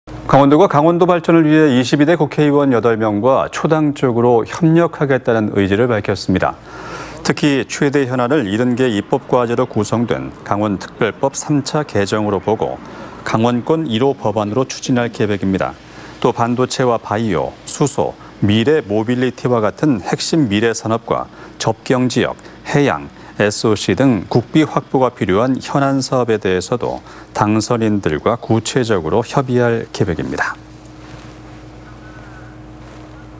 강원도 "22대 국회의원 8명과 초당적 협력"
강원도가 강원도 발전을 위해
22대 국회의원 8명과 초당적으로
협력하겠다는 의지를 밝혔습니다.

 특히, 최대 현안을 70개의 입법과제로 구성된
강원특별법 3차 개정으로 보고
강원권 1호 법안으로 추진할 계획입니다.

 또 반도체와 바이오, 수소,
미래모빌리티와 같은 핵심 미래산업과
접경지역, 해양, SOC 등
국비 확보가 필요한 현안 사업에 대해서도
당선인들과 구체적으로 협의할 계획입니다.